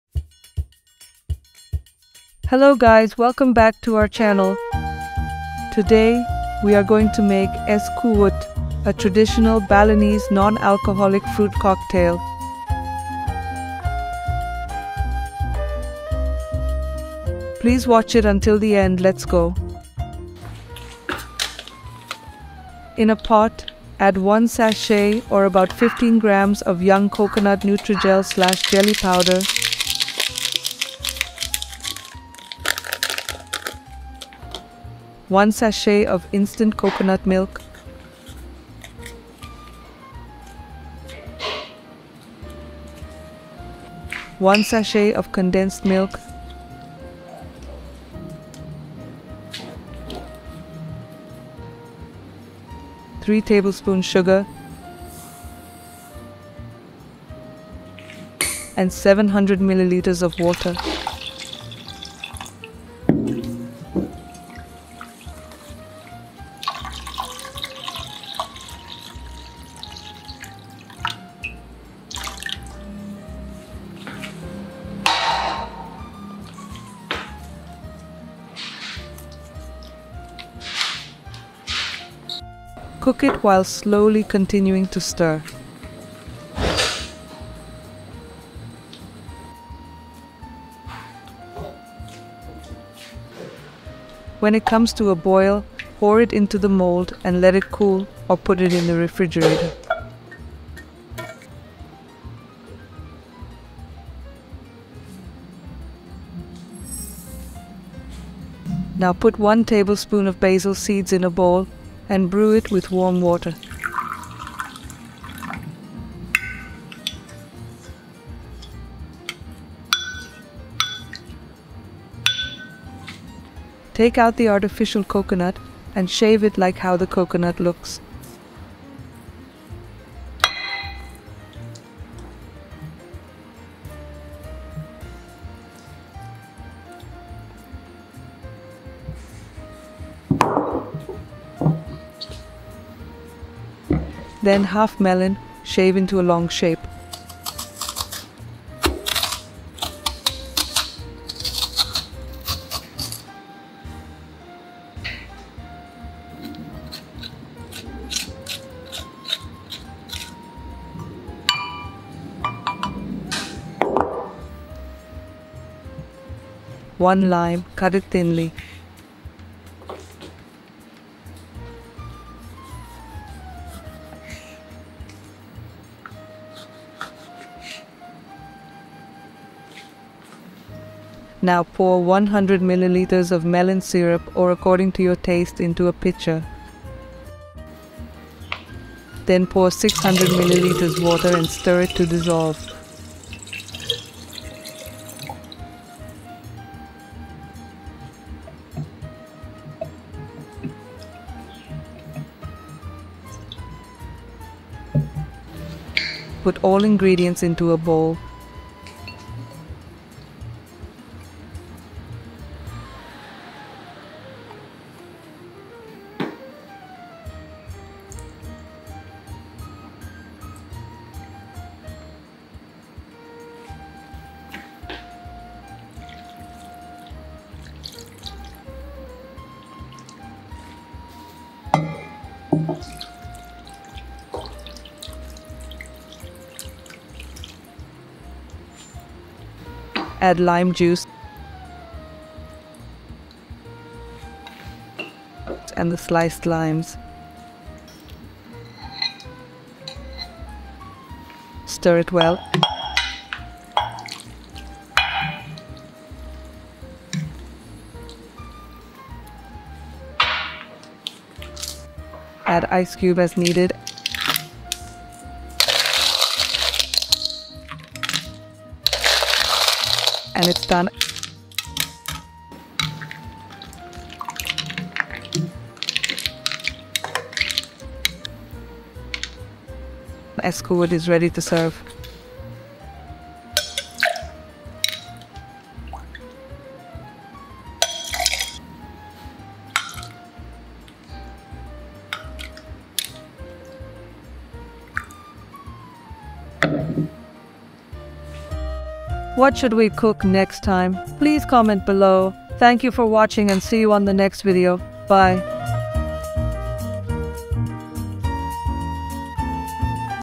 Experience the Freshness and Sweetness of Es Kuwut During Iftar | Simple and Easy Recipe
SAVE THIS VIDEO FOR IFTAR, FRESH, SWEET, AND COLD KUWUT  ARTIFICIAL YOUNG COCONUT ICE
ARTIFICIAL YOUNG COCONUT

 INGREDIENTS:
1 SACHET OF YOUNG COCONUT NUTRIJEL (15 GR)
40 GR  CONDENSED MILK
3  TBSP SUGAR
1 SACHET OF INSTANT COCONUT MILK
700ML WATER
OTHER INGREDIENTS:
HALF OF MELON
1 TBSP SELASIH
1 LIME, Thinly CUT
1 LIME JUICE
100 ML MELON SYRUP / according to taste
600ML WATER
 ICE Cubes
 MINT LEAVES

METHOD:
1 . MIX ALL YOUNG COCONUT ARTIFICIAL INGREDIENTS IN A POT, MIX  PERFECTLY ALL INGREDIENTS,  THEN COOK WHILE COOKING SLOWLY CONTINUES TO MIX, AFTER BOILED TURN OFF STOVE CONTINUOUSLY  MIXING UNTIL THE STEAM IS Disappear. POUR INTO THE MOLD, LET SET  CAN ALSO PLACE IN THE REFRIGERATOR.
2. BREW SELASIH WITH WARM WATER SET ASIDE
3. SHAVED  MELON TO THE LONG SHAPE
4. SHAVED ARTIFICIAL COCONUT TO THE LONG SHAPE
5. Thinly SLICES LIME
6. Mix melon syrup with water
7. IN A BOWL MIX ALL THE INGREDIENTS
8. TO SERVE ICE PUT SOME ICE CUBES IN THE GLASS AND OTHER INGREDIENTS
9. GARNISH WITH MINT LEAF

See Our behind the Scene and other video at